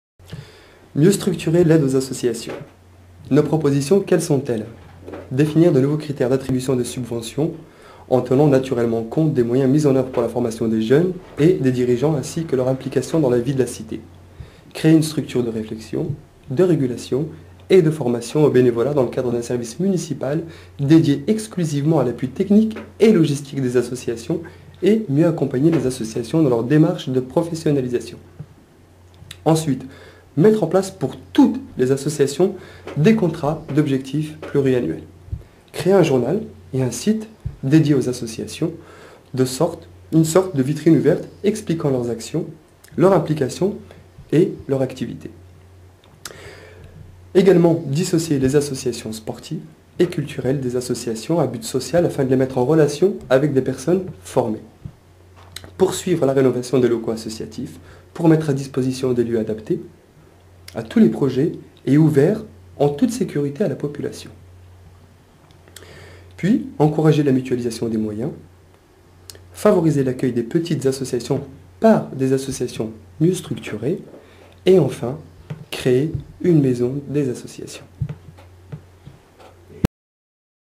Evreux avant tout - Programme par Driss Ettazaoui
Driss Ettazaoui responsable politique au sein d'une collectivité territoriale, précise, ici, l'intérêt de mieux structurer l'aide aux associations;